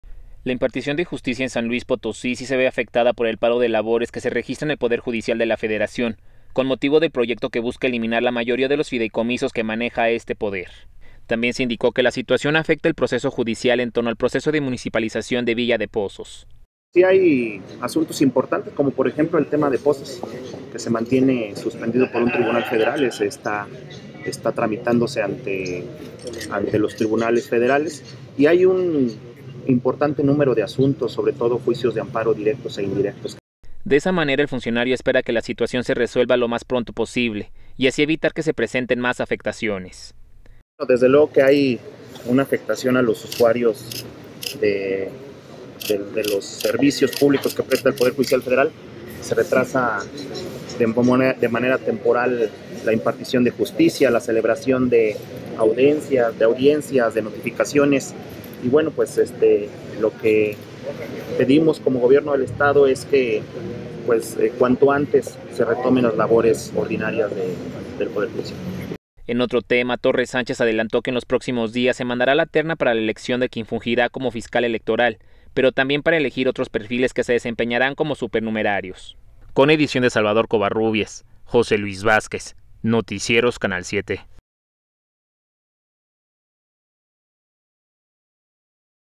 Asuntos importantes, suspendidos debido al paro en Poder Judicial de la Federación: J. Guadalupe ...
Asuntos importantes, suspendidos debido al paro en Poder Judicial de la Federación: J. Guadalupe Torres

SLP.- La impartición de justicia en San Luis Potosí sí se ve afectada por el paro de labores que se registra en el Poder Judicial de la Federación con motivo del proyecto que buscar eliminar la mayoría de los fideicomisos que maneja este poder.

También se indicó que la situación afecta el proceso judicial entorno al proceso de municipalización de Villa de Pozos.

"Hay asuntos importantes como por ejemplo el tema de Pozos que se mantiene suspendido por un Tribunal Federal y se está tramitando ante los tribunales federales y hay un importante número de asuntos sobre todo juicios de Amparo directos e indirectos", expresó el secretario general del gobierno, Guadalupe Torres Sánchez.

De esa manera, el funcionario espera que la situación se resuelva lo más pronto posible y así evitar que se presenten más afectaciones

"Desde luego que hay una afectación a los usuarios de los servicios públicos que ofrece el Poder Judicial Federal, se retrasa de manera temporal la impartición de justicia, la celebración de audiencias de notificaciones; lo que pedimos como gobierno del estado es cuanto antes se retomen las labores ordinarias del poder judicial", culminó.

En otro tema, Torres Sánchez adelantó que en los próximos días se mandará la terna para la elección de quien fungirá como Fiscal electoral, pero también para elegir otros perfiles que se desempeñarán como supernumerarios.